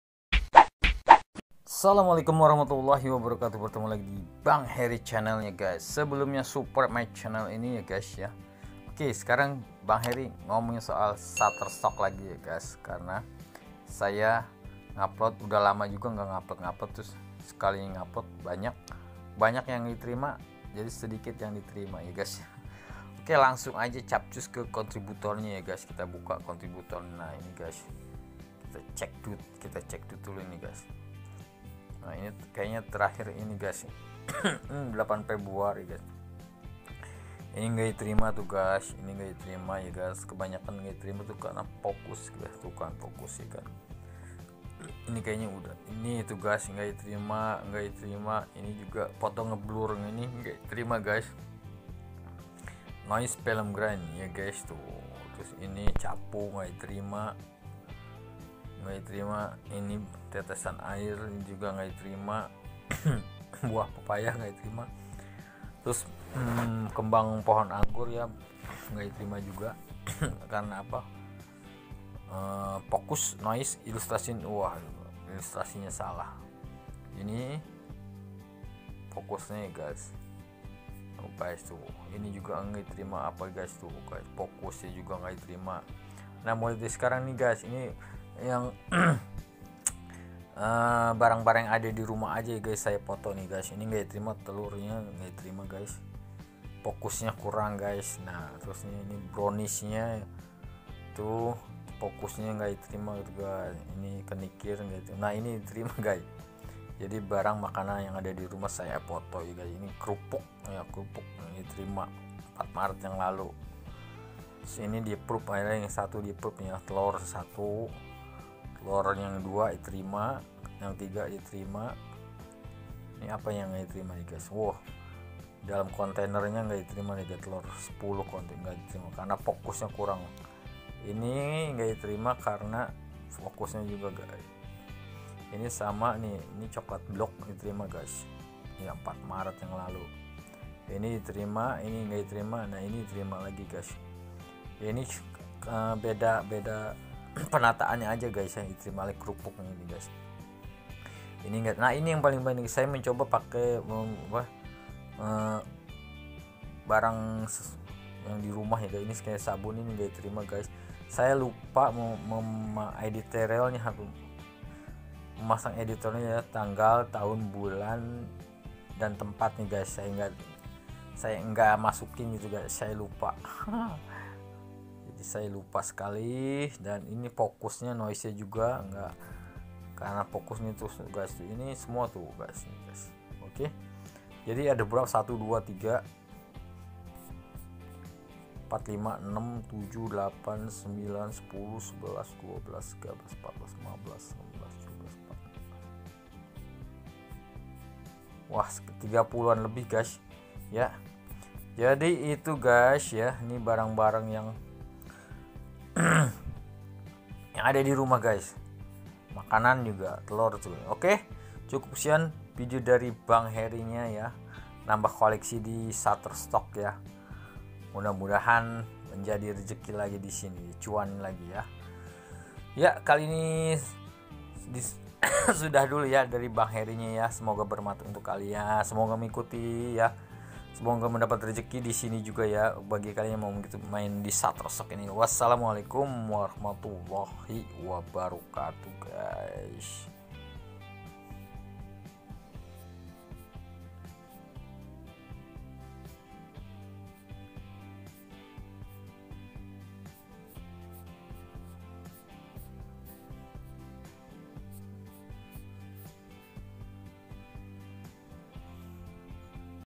barang apa aja,foto apload di shutterstock
foto yg ada di rumah..apload di shutterstock syukur syukur approved